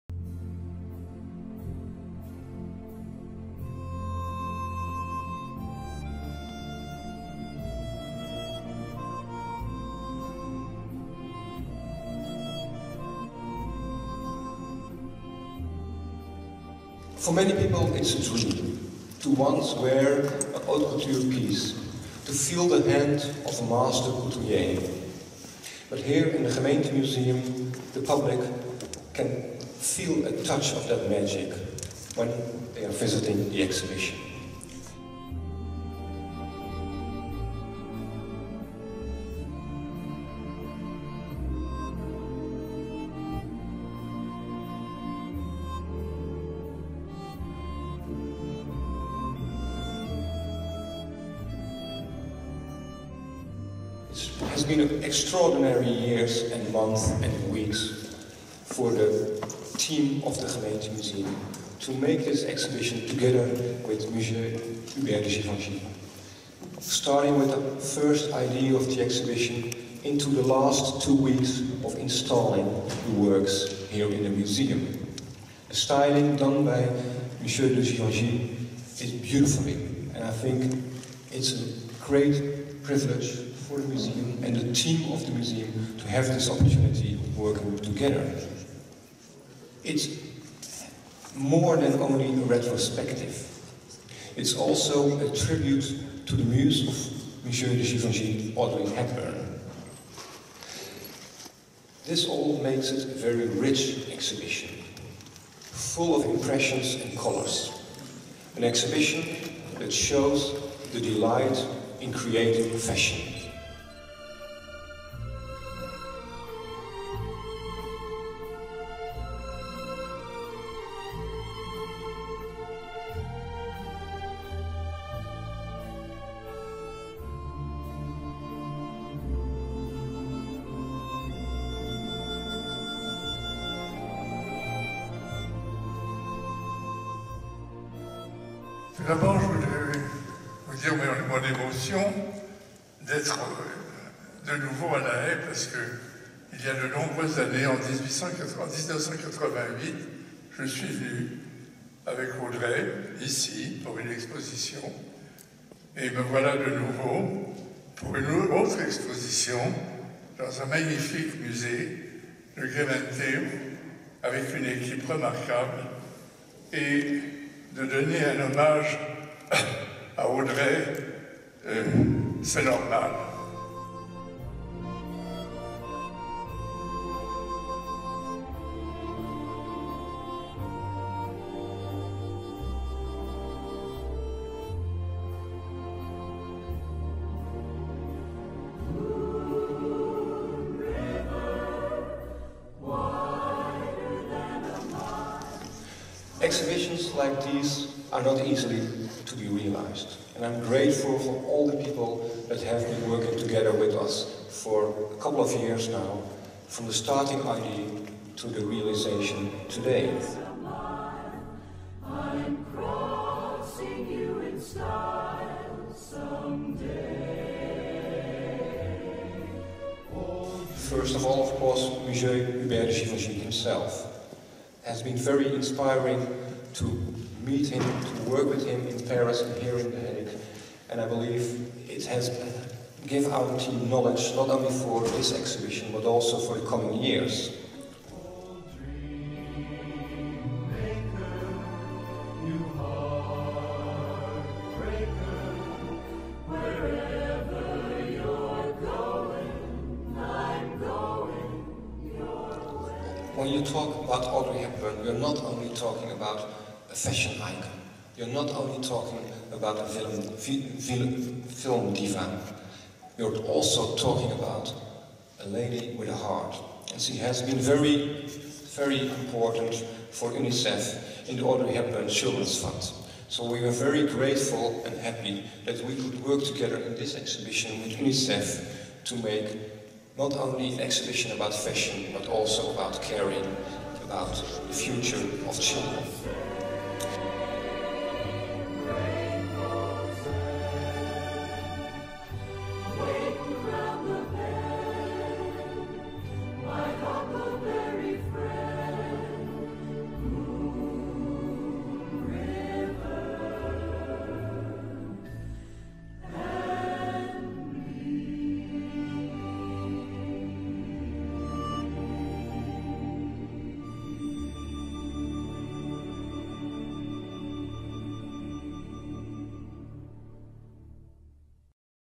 Tentoonstelling Hubert De Givenchy — Haags Gemeente Museum
De Franse couturier Hubert de Givenchy is een van de grote modeontwerpers van de 20ste eeuw. Hij werkte in dezelfde tijd als Christian Dior en Cristóbal Balenciaga en is dus met recht een levende legende in de geschiedenis van de haute couture. In nauwe samenwerking met Monsieur De Givenchy zelf presenteert het Gemeentemuseum een groots opgezet retrospectief van zijn oeuvre én een eerbetoon aan zijn prachtige muze Audrey Hepburn.

Hubert De Givenchy – To Audrey with Love geeft een uniek inzicht in De Givenchy’s werk dat de tijdspanne van een halve eeuw omvat, van de opening van Maison de Givenchy in 1952 tot zijn terugtreden bij het modehuis in 1995. De couturier zelf heeft talrijke van zijn meest geliefde creaties geselecteerd, waarvan sommigen nog nooit eerder voor publiek werden tentoongesteld. Bovendien zal de tentoonstelling deels speciaal gewijd zijn aan de bijzondere vriendschap en samenwerking tussen Monsieur De Givenchy en Audrey Hepburn. De tentoonstelling zal schitterende ontwerpen tonen van De Givenchy, deels gedragen door Audrey Hepburn, maar ook ontwerpschetsen, tekeningen, foto’s en filmbeelden.

Audrey Hepburn was mooi van buiten én van binnen. In samenwerking met UNICEF Nederland wordt een zo compleet mogelijk beeld geschetst van deze legendarische actrice, muze van De Givenchy en tot op de dag van vandaag een inspiratiebron voor velen.